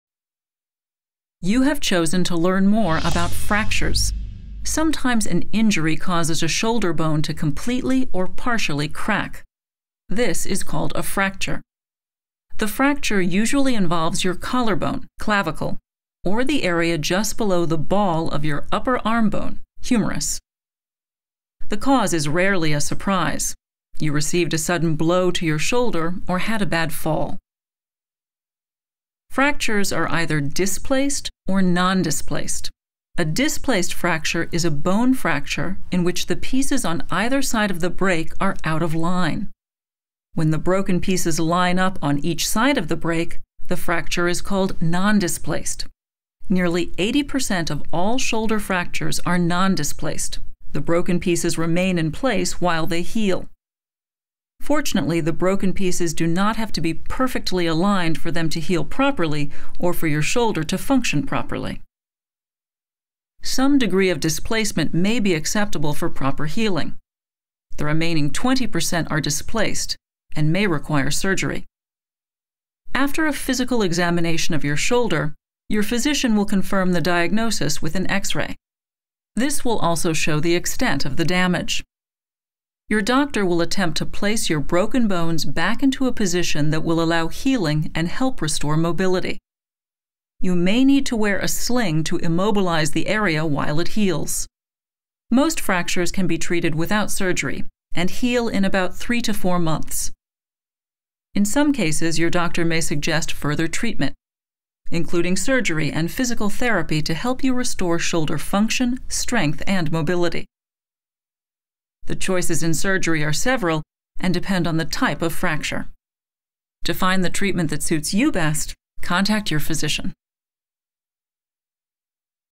What You Need to Know About Shoulder Fractures and Treatment | DePuy Synthes
Shoulder fractures usually involve the clavicle or the humerus. They can be displaced or non-displaced, and treatment varies depending on the fracture. Watch to learn more about how these injuries are managed.

0:00 Introduction to Shoulder Fractures
0:12 Common bones involved: clavicle & humerus
0:29 Types of fractures: displaced vs non-displaced
0:46 Healing of non-displaced fractures
1:13 Diagnostic process: physical examination and X-ray scan
1:23 Treatment overview: bone realignment and immobilization
1:35 Shoulder fracture healing time and rehabilitation

Watch this video and learn about shoulder fractures, including their treatment and recovery process.